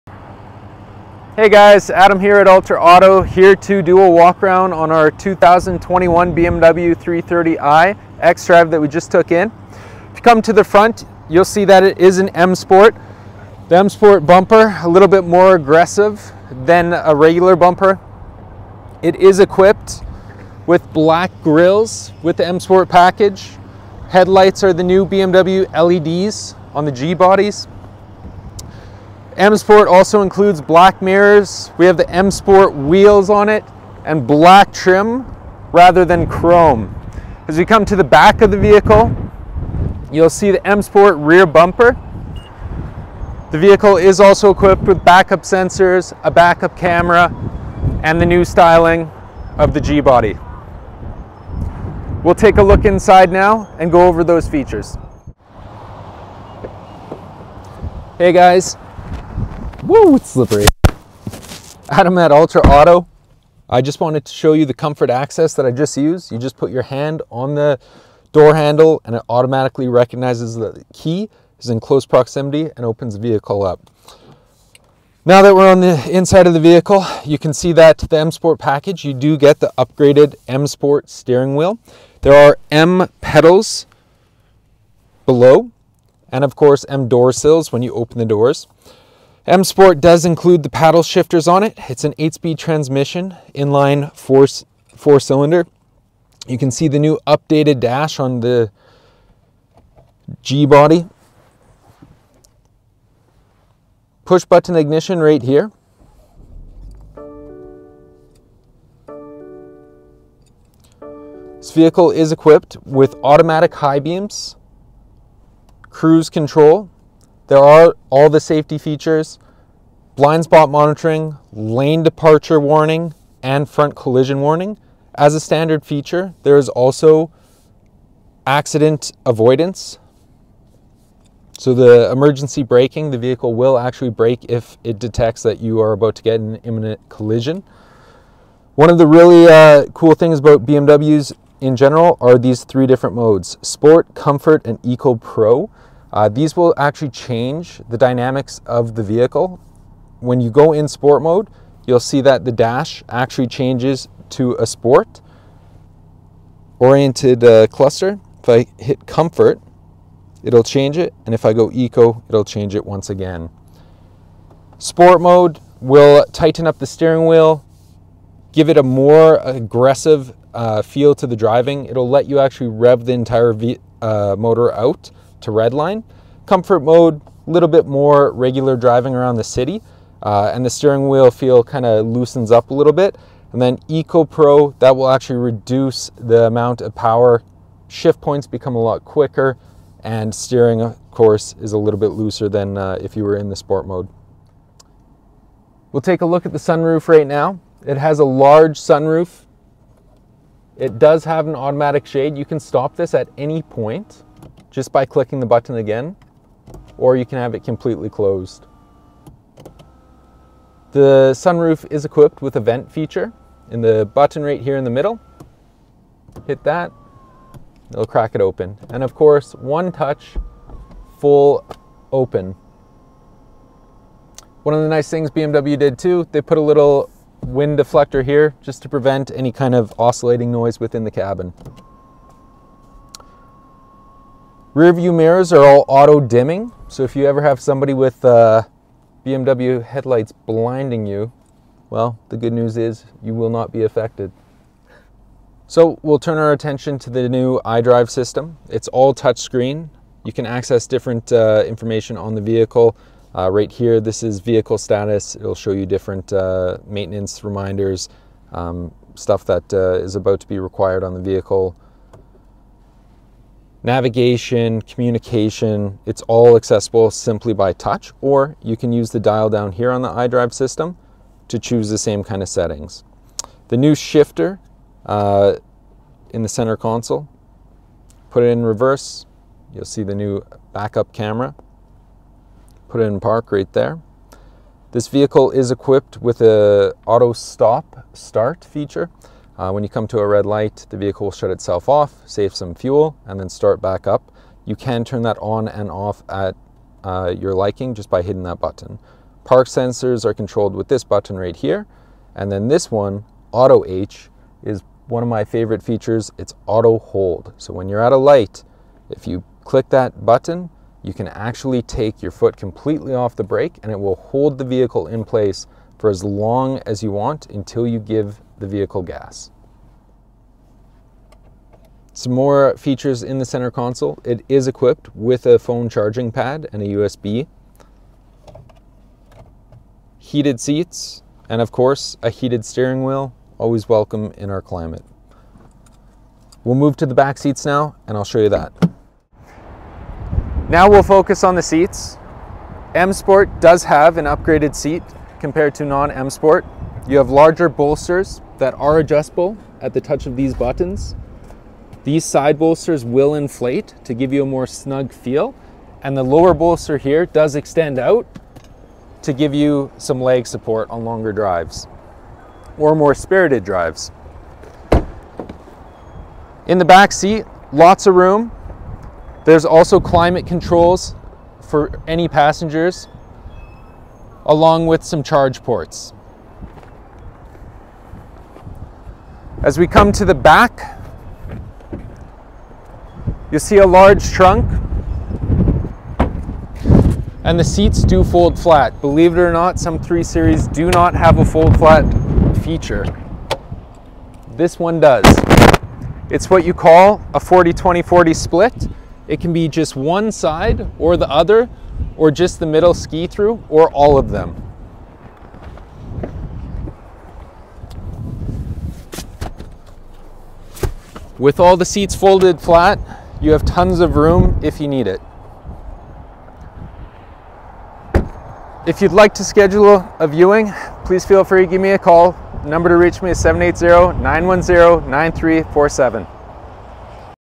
2021 BMW 3 Series 330i xDrive | Stock M0679 | UltraAuto
Check out this 2021 BMW 3 Series 330i xDrive available here at Ultra Auto.

330i xDrive | M SPORT | Premium Pkg Essential | WLAN Hotspot | Heated Memory Seats | Sunroof | NAV | Sedan 8-Speed Automatic w/OD

Stock

Ultra Auto
14710 Mark Messier Trail NW
Edmonton, AB T6V 1H5
Canada